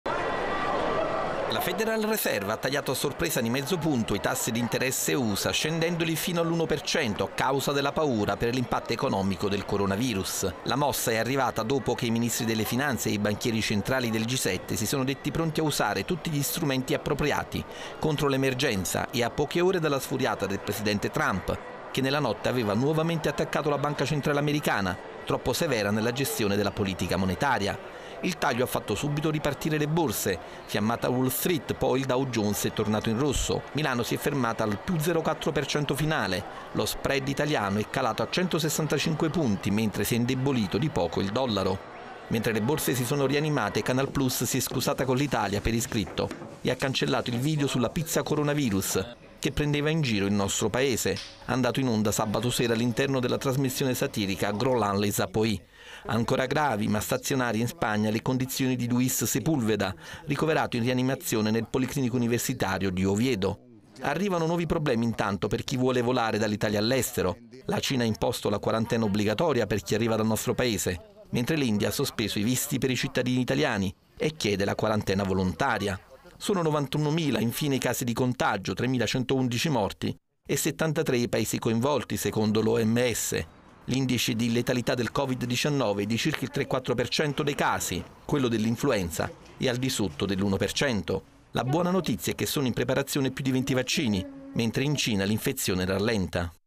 Coronavirus, nel mondo superati i 90 mila contagi. India blocca visti agli italiani
Superati nel mondo i 90mila contagi da Covid 19.  L’India ha bloccato i visti anche agli italiani. Intanto i ministri finanziari del G7 assicurano l’impegno per  salvaguardare la crescita economica.  Servizio di Luigi Ferraiuolo